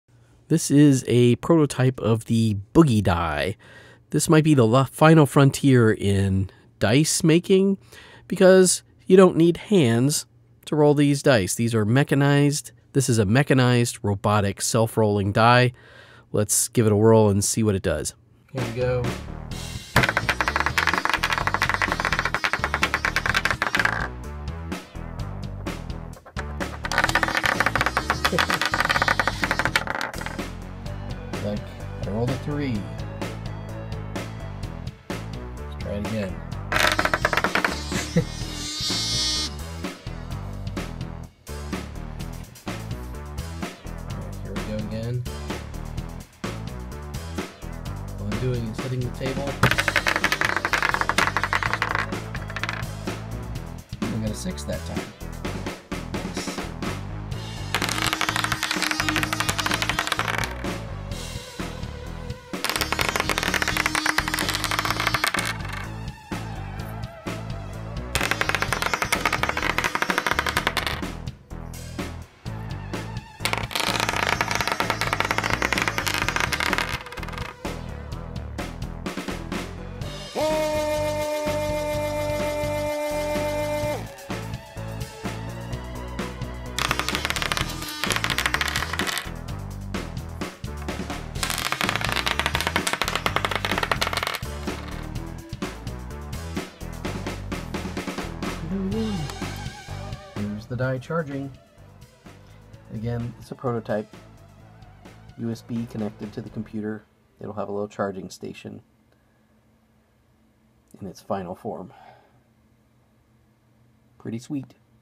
Boogie Dice: Self-Rolling Mechanized Dice
A brief test of a prototype Boogie Die, a self-rolling robotic mechanized die. The dice respond to sound or vibration or a short drop to roll themselves. The finished product will be programmable.